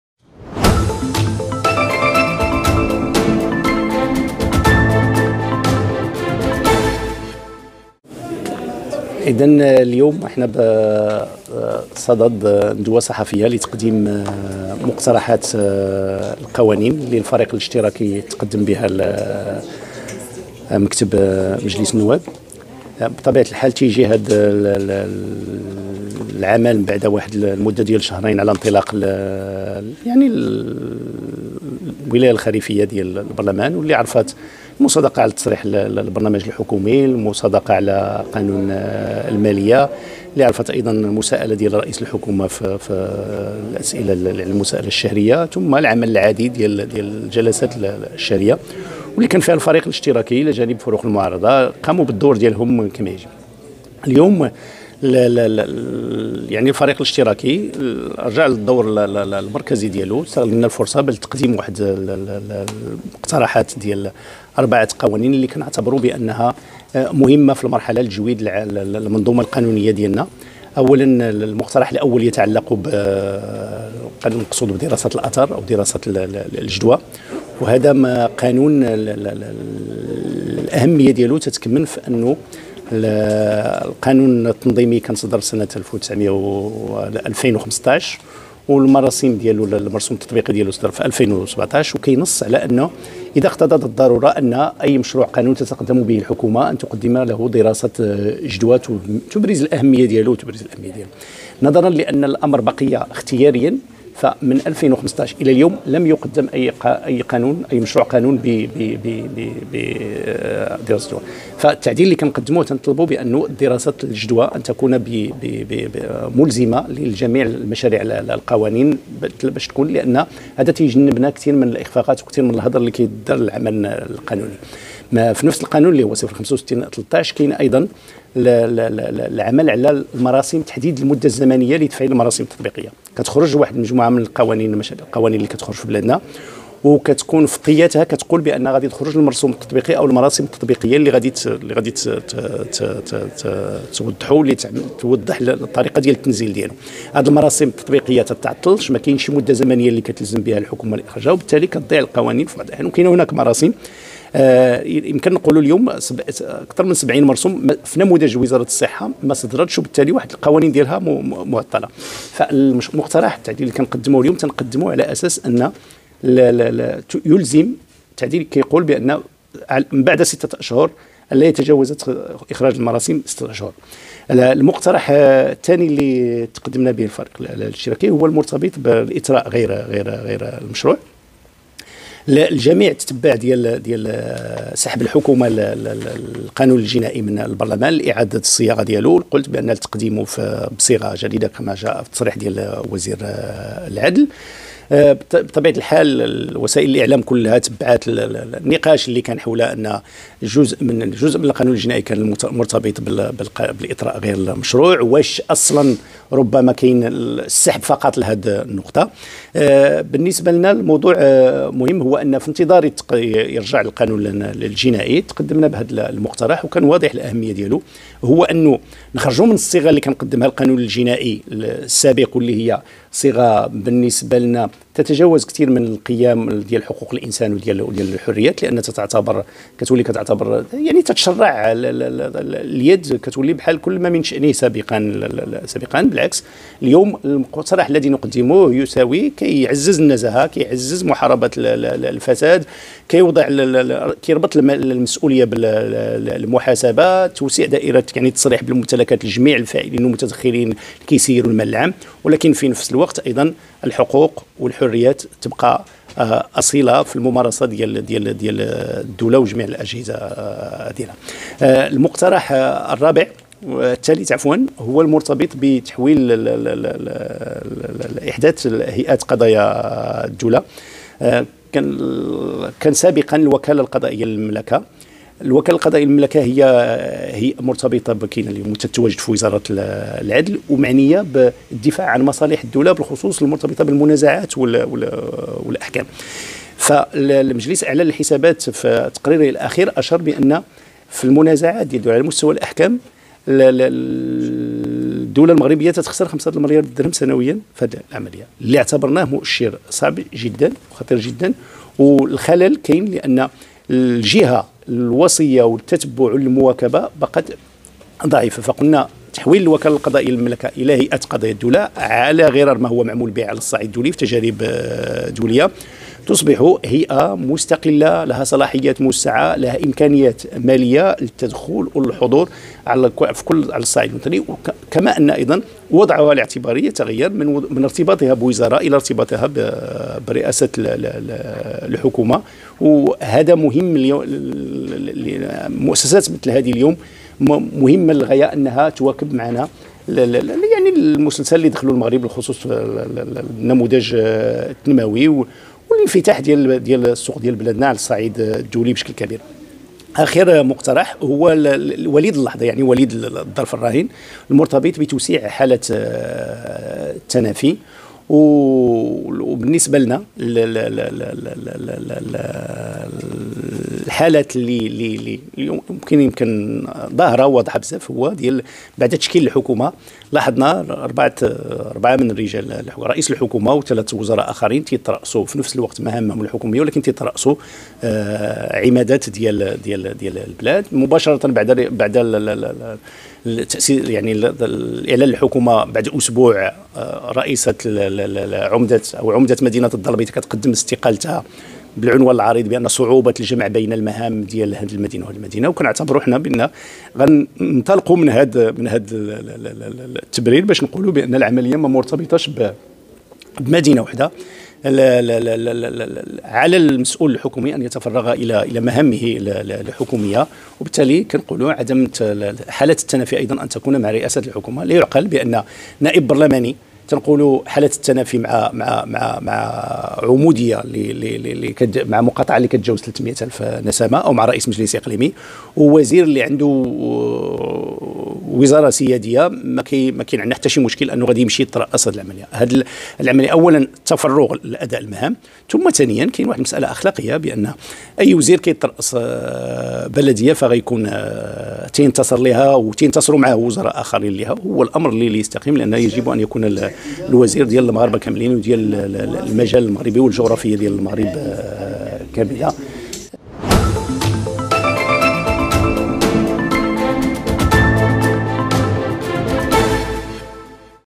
شهيد يستعرض مقترحات قوانين الفريق الاشتراكي بمجلس النواب
أبرز النائب عبد الرحيم شهيد، رئيس الفريق الاشتراكي أن المقترحات التشريعية للفريق الاشتراكي تتوخى الرفع من جودة التأطير القانوني لتكريس الحكامة العمومية، وتوفير الآليات الضرورية لتخليق مجال تدبير الشأن العام وتقييم السياسات العمومية.

# أخبار_عاجلة